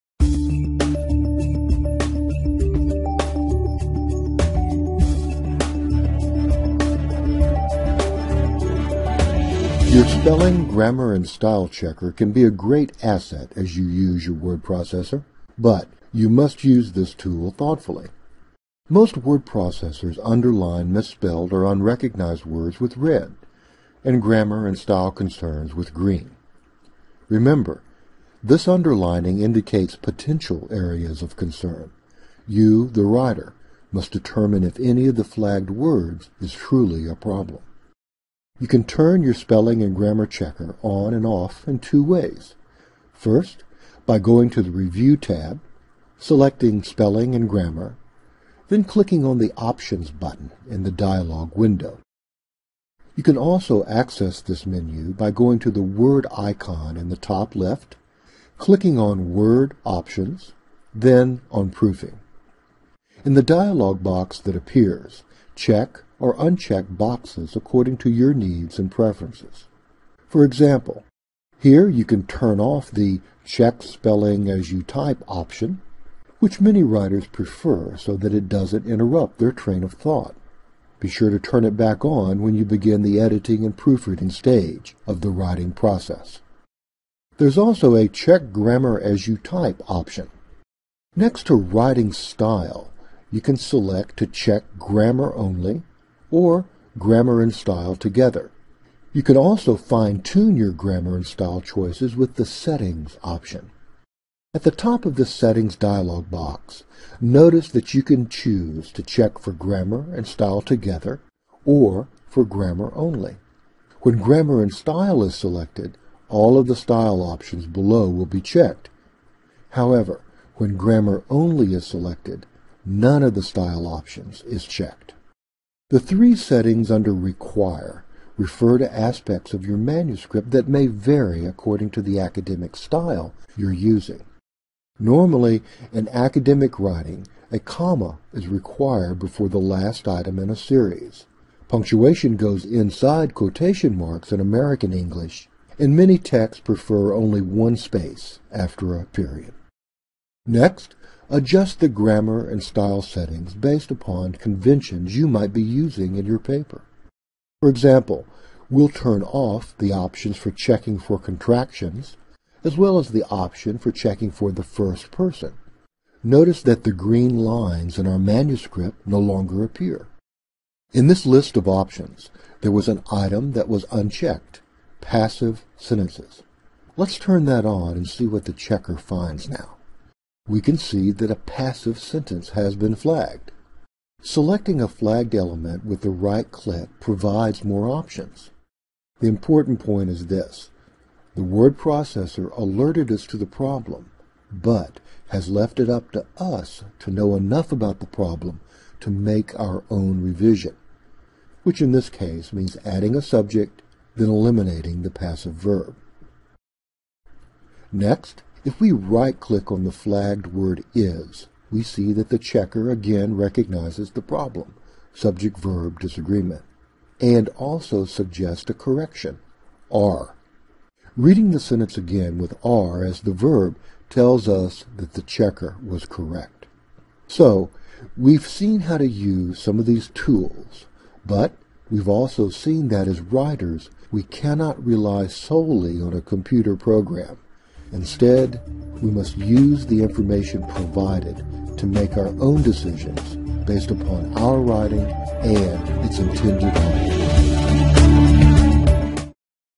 Spelling & Grammar Checker - Word
How to set up and use Word's spelling and grammar checker.